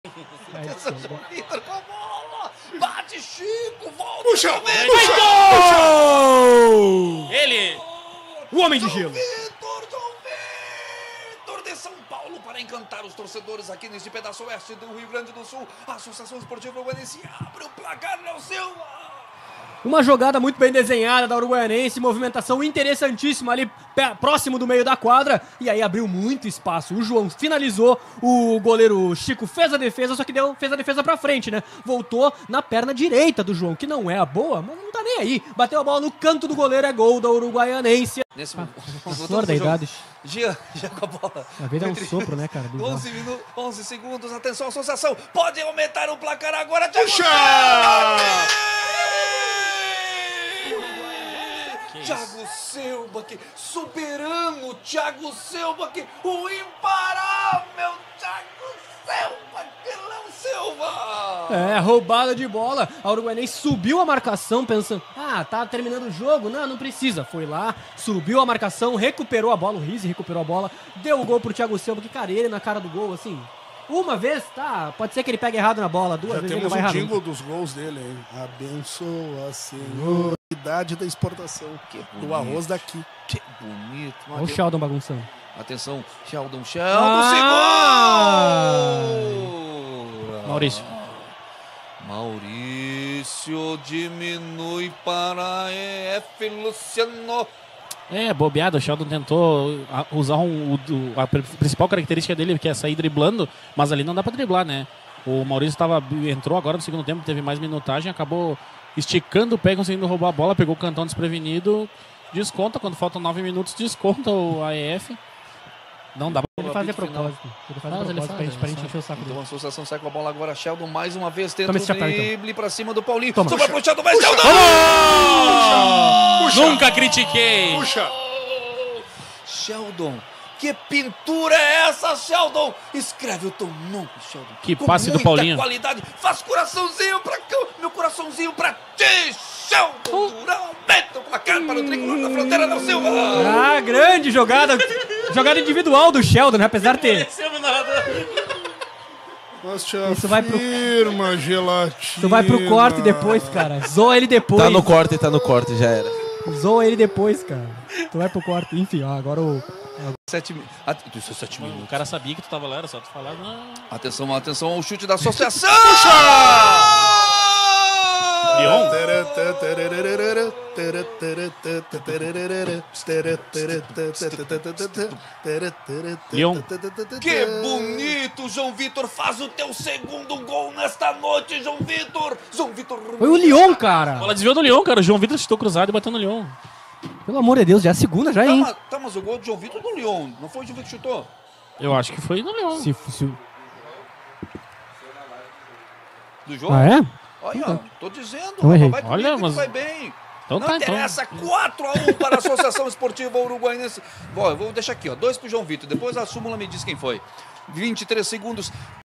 Confira os gols do primeiro jogo da semi final da Copa dos Pampas entre AEF 1 X 4 AEU.
Gols:
⚽Joao Melges
⚽Tiago Selbach
⚽Mauricio
⚽Sheldon Durand
⚽Lion Ribeiro

Creditos das Imagens : Rádio Santo Angelo